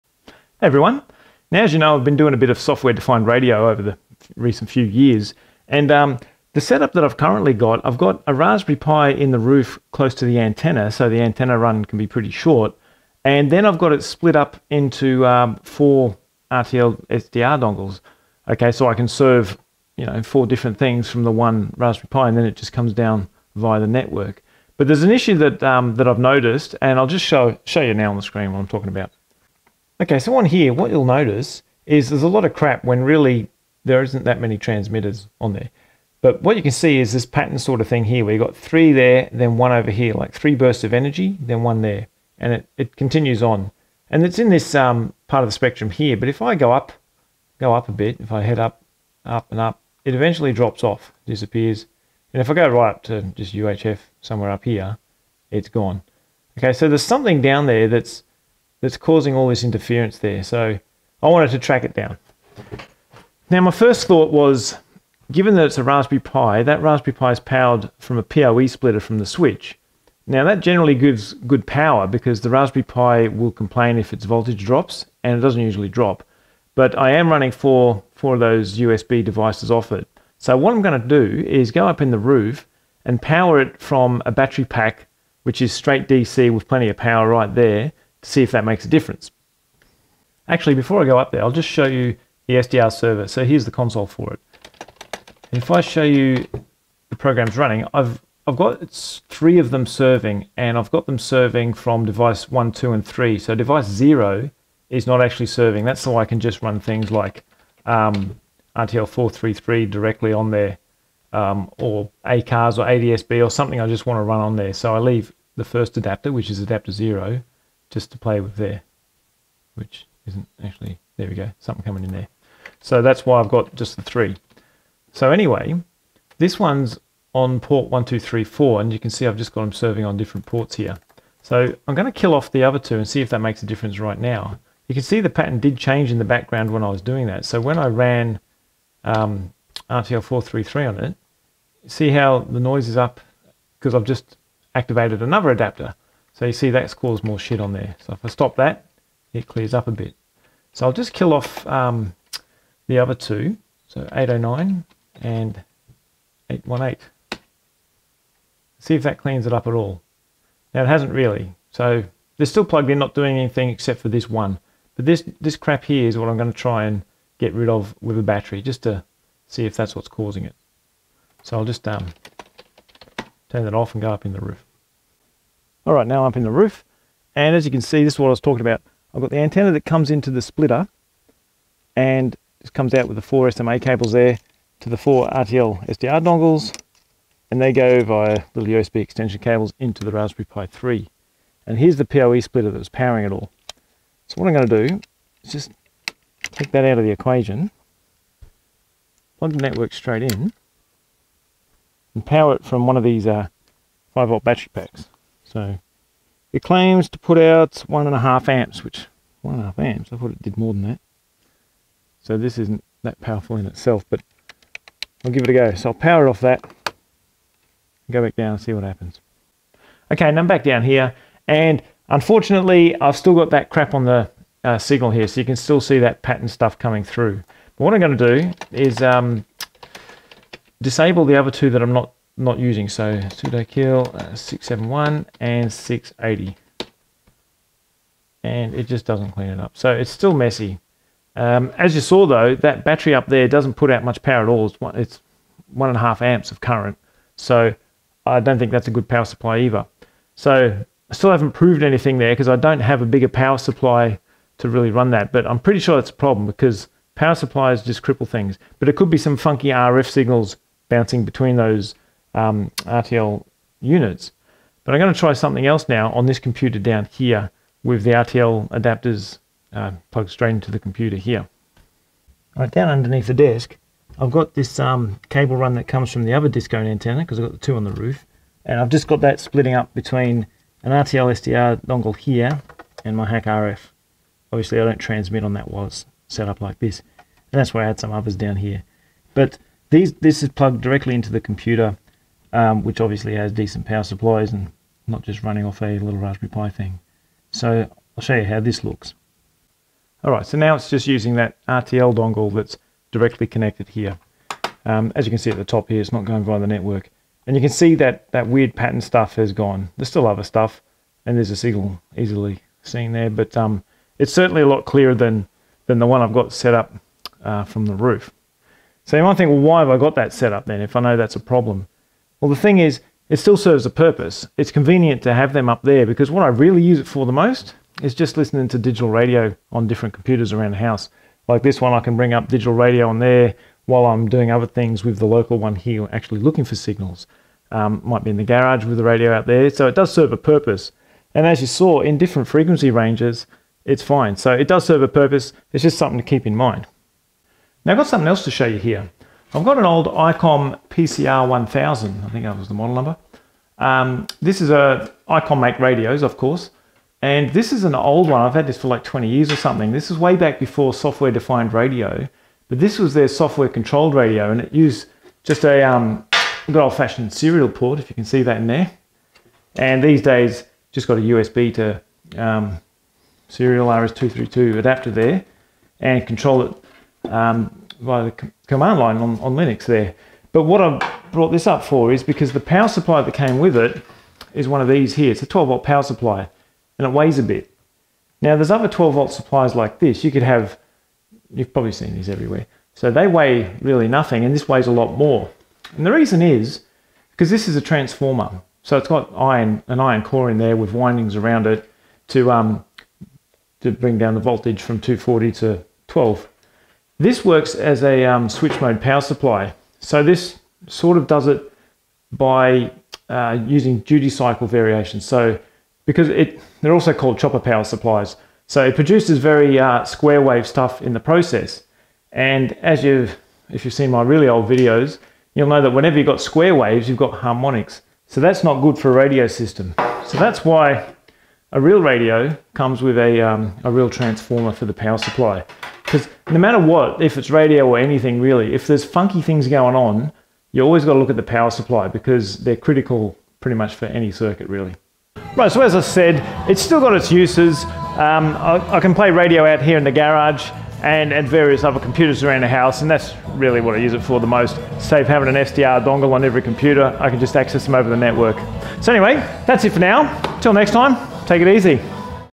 Radio Power Supply
The importance of a good power supply for radio systems, and any other type of system too.